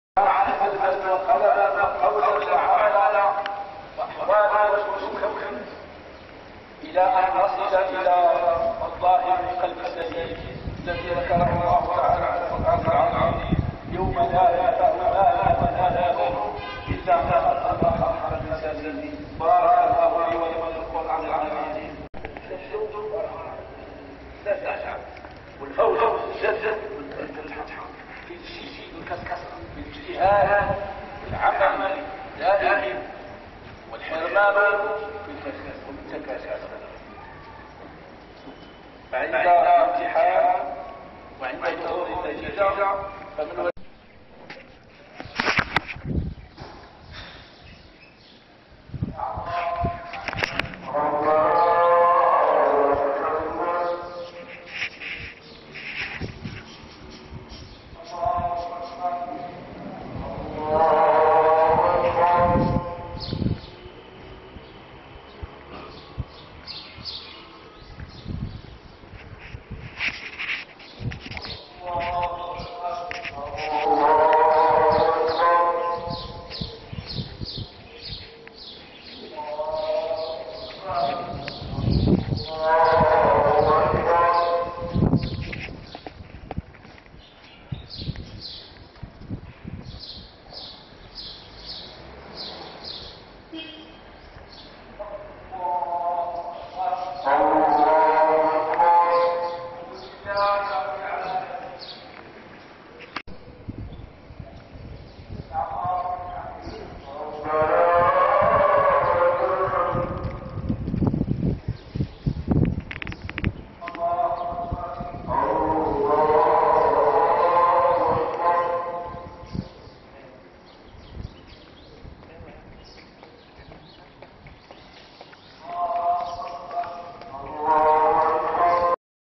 ► Casablanca (prayer-الصلاة)
Prayer in the streets of Casablanca, Morocco, June 5, 2009
الدار البيضاء
al-Mamlaka al-Maġribiyya
الصلاة
الله أكبر

See also
Kuala Lumpur-Kota Kinabalu | prayer | doa wmv